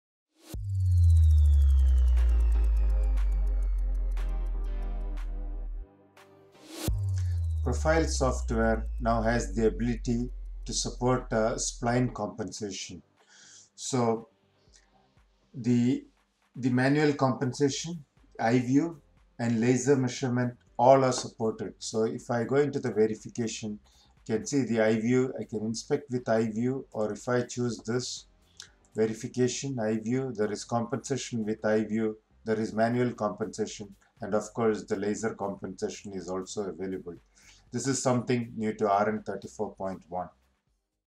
Tool Tip Tuesday #132 - Support for Spline Compensation
ANCA's profile software now has the ability to support spline compensation with manual compensation, iView and laser measurement. You can find these under the Verification tab in the latest RN34 version of ToolRoom.